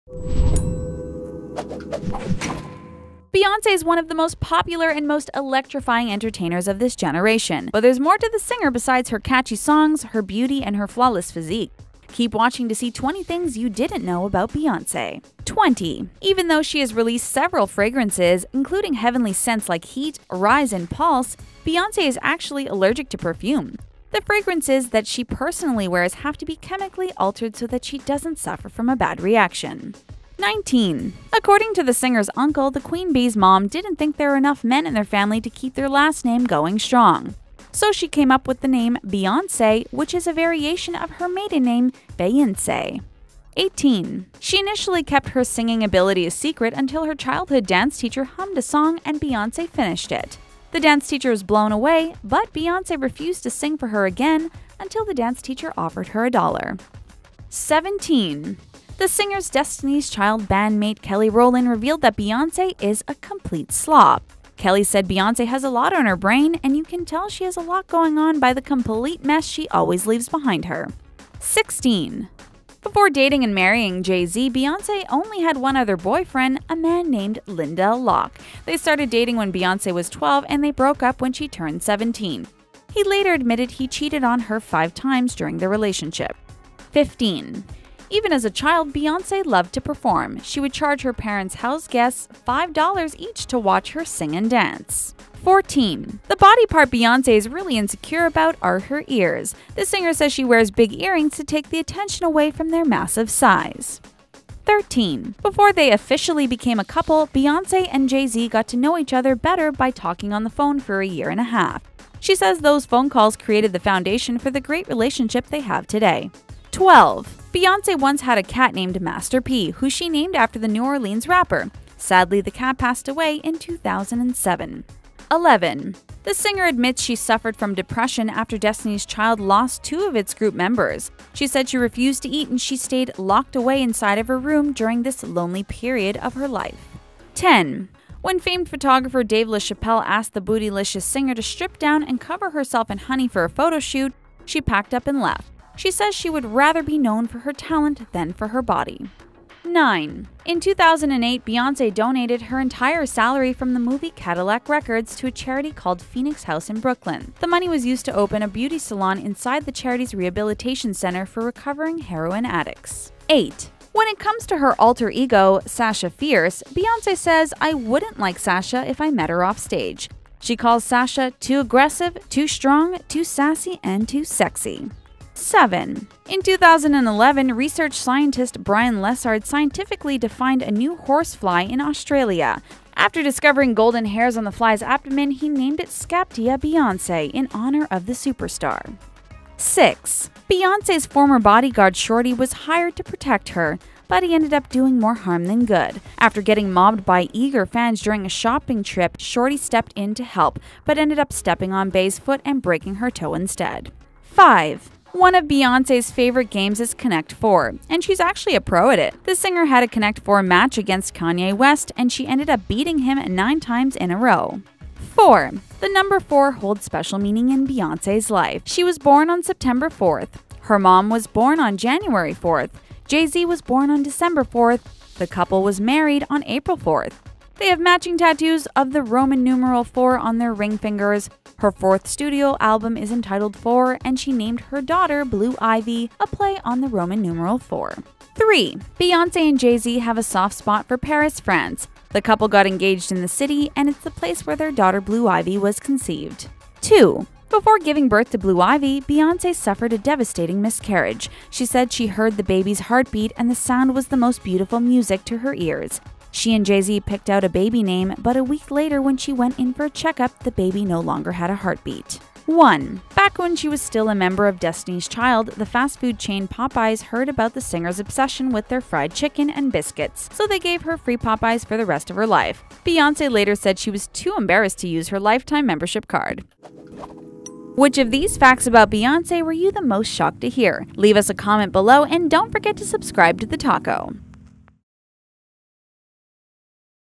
20 Things You Should Know About Beyoncé
20 Quick facts you didn't know about Queen Bey.

Description:
Beyoncé is undeniably one of the top entertainers of this generation. She has won numerous Grammys, has traveled all over the world, has sold over 100 million records, and has left us with some of the most memorable Super Bowl and MTV awards show performances. When Beyoncé initially came out on the music scene, she tried extremely hard to keep her private life under wraps, and she succeeded for many years. But now, she is a little more open, and she has revealed bits and pieces about herself that we never even knew.